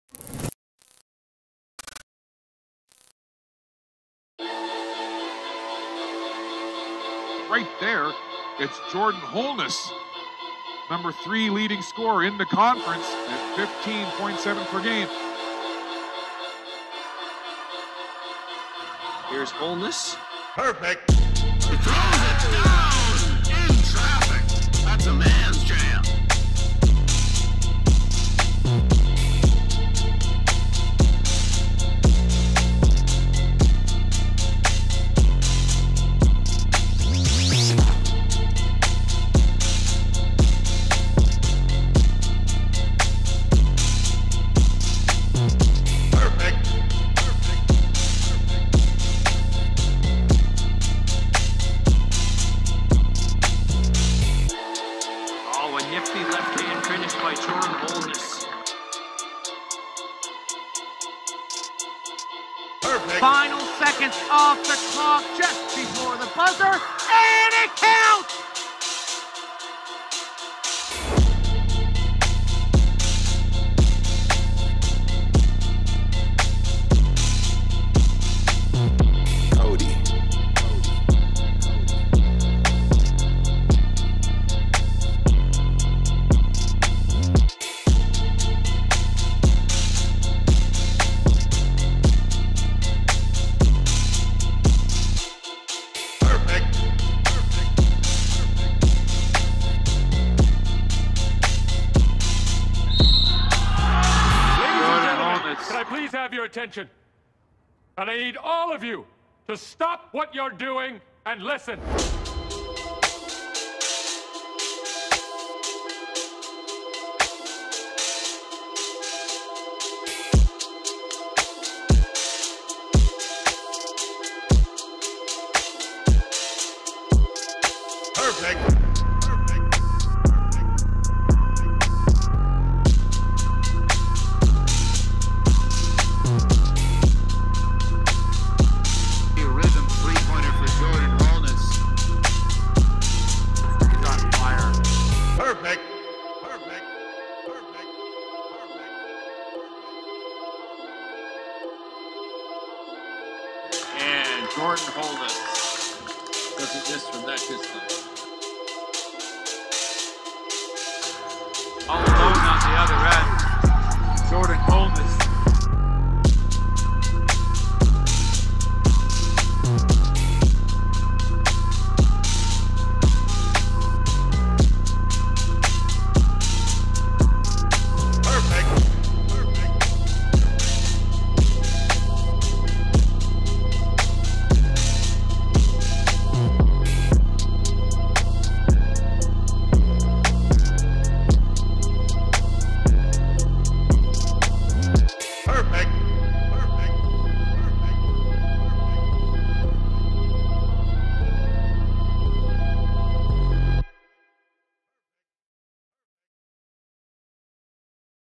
#7 from the 6ix | Jordan Holness '18-'19 Holland Hurricanes Season Highlights (Beat Prod. by Cxdy)
Jordan Holness is a POINT GOD. In his first season with the Holland College Hurricanes, the Toronto native showed off his ridiculously smooth game helping the 'canes win their 7th consecutive ACAA gold medal averaging 16 ppg. His highlights include everything for threes, dunks, flashy assists, handles, defensive plays, and jumpers from anywhere on the court.

Shout out to Cdxy for the beat (@cdxy)

Follow All Eyes East:
Facebook: All Eyes East Productions